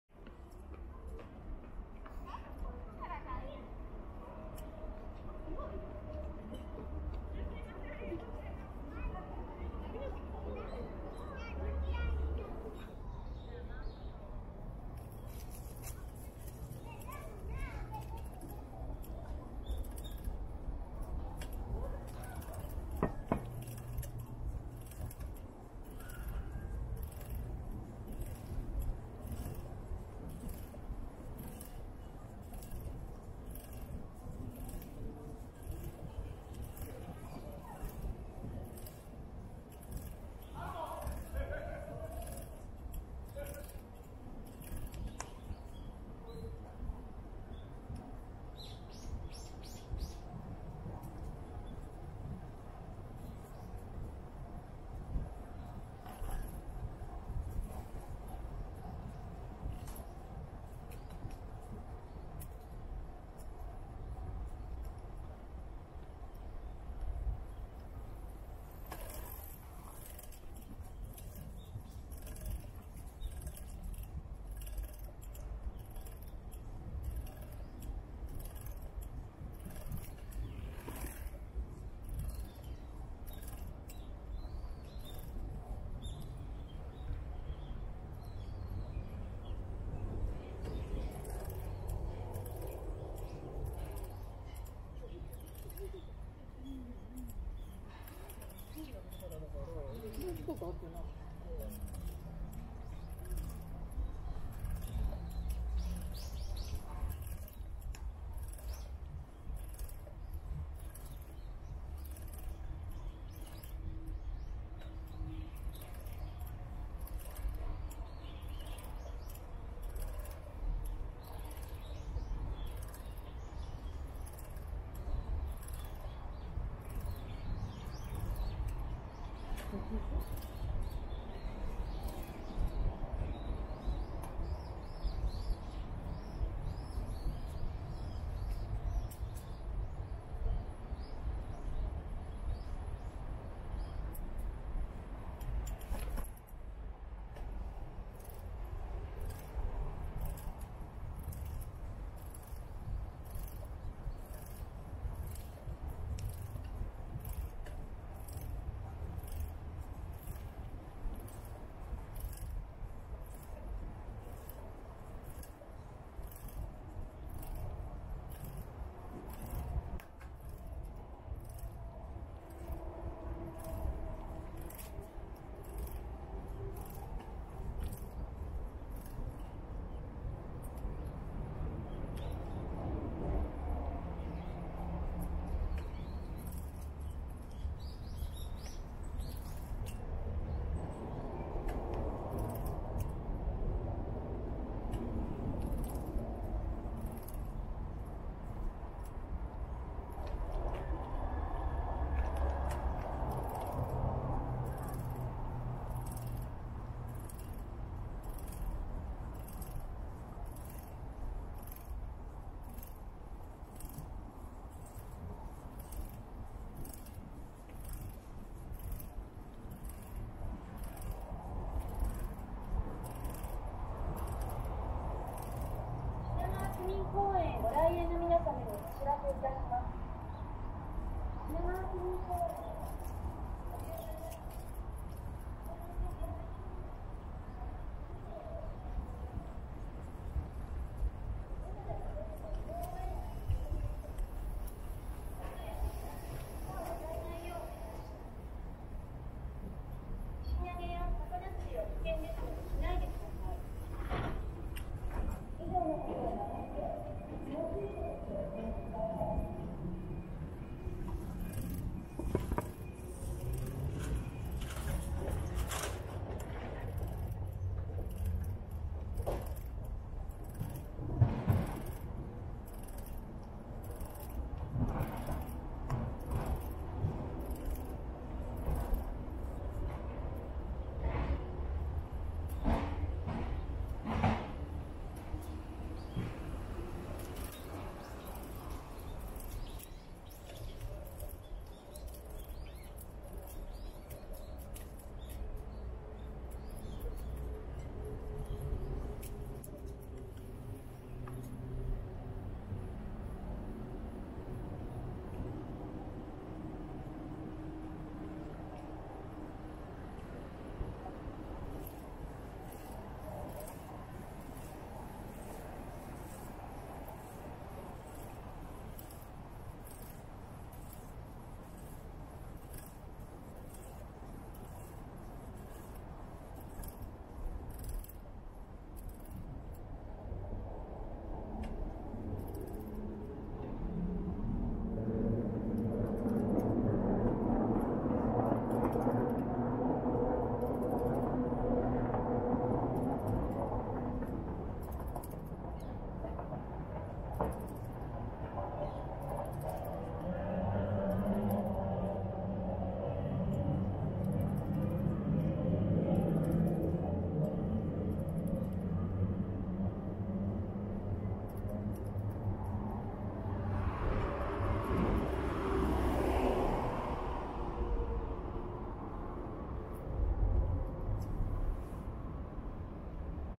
自転車散歩 「しながわ区民公園 サイクリングコース」を走る
前回、OSMO POCKET をハンドルに取り付けて動画撮影したところ、段差や凸凹で、画面が乱れました。今回から、バックパックのストラップにつけるという取り付け方法に変えて撮影しました。マイク部分にスポンジを貼り付けましたので、風切り音もかなり低減できています。
画像の乱れる動画はこちら
しながわ区民公園は、歩行者通路と自転車道が明確に分かれているのはいいのですが、表記が少ないので、自転車道を歩く人が多くて、ちょっと走りにくいですね。